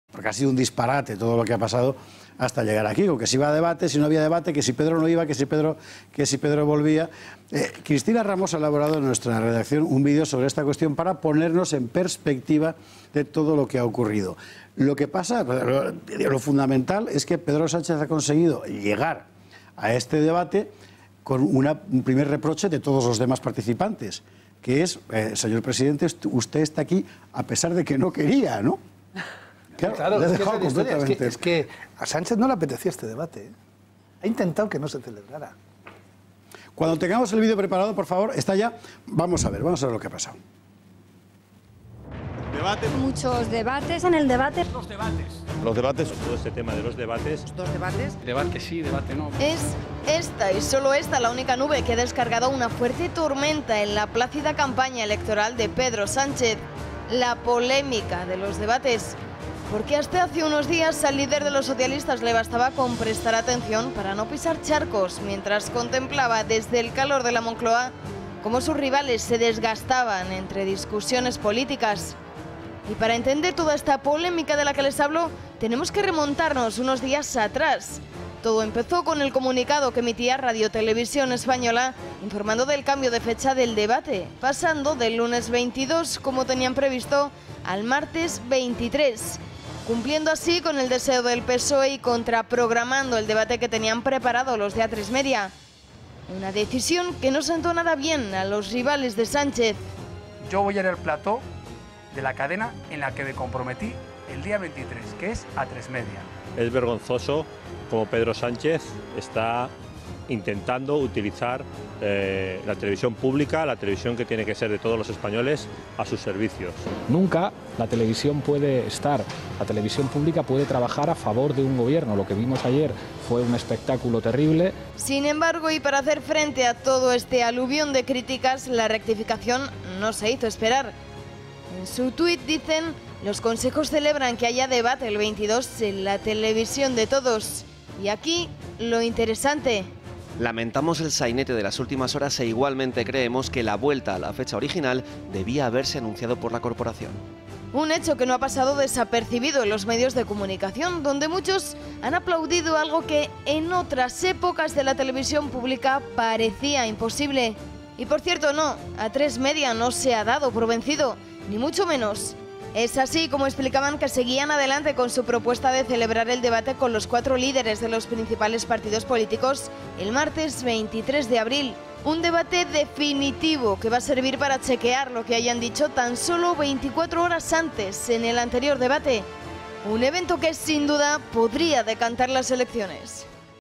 El debate que Sánchez quiso evitar que demuestra que TVE está al servicio del socialismo
¿Manipula a su antojo Rosa María Mateo para favorecer a Pedro Sánchez?

Es evidente. Con Pedro Sánchez RTVE está al servicio del PSOE. El intento frustrado de Pedro Sánchez por cambiar la fecha del debate del 22 al 23 de abril para contraprogramar el de Atresmedia ha sido el último espectáculo bochornoso protagonizado por Rosa María Mateo de la mano de Pedro Sánchez. Aunque la manipulación del debate sólo quedó en un intento frustrado, sirvió para demostrar que la corporación pública tan sólo es un títere más de Pedro Sánchez ¿Tiene miedo el líder del PSOE a los debates? ¿Se ha pasado esta vez Sánchez moviendo hilos para favorecer los intereses del PSOE? Profundizamos en la polémica de los debates, vean.